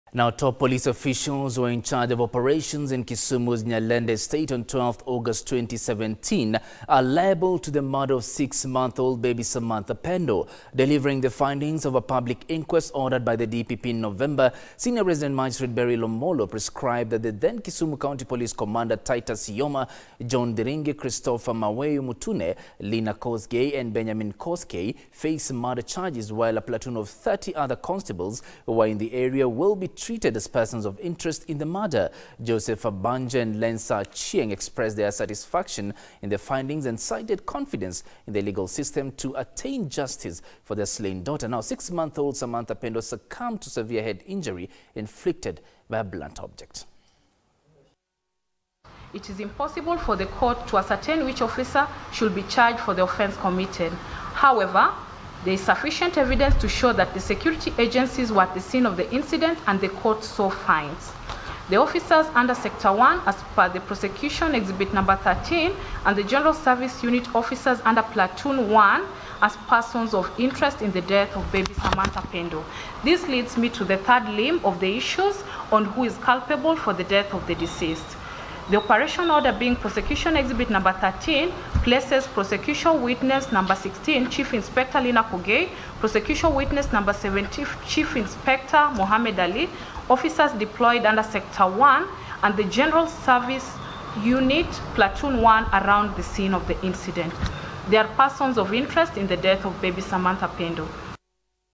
Kisumu court rules police culpable on baby Pendo murder
Top police officials who were in charge of operations in Kisumu's Nyalenda estate on 12th August 2017 are liable for the murder of six month old Baby Samantha Pendo. Delivering the findings of a public inquest ordered by the DPP in November, senior resident magistrate Beryl Omolo, prescribed that the then Kisumu county police commander Titus Yoma, John Thiringi, Christopher Maweu Mutune, Lina Kosgei and Benjamin Koskey face murder charges while a platoon of 30 other constables who were in the area will be treated as persons of interest in the murder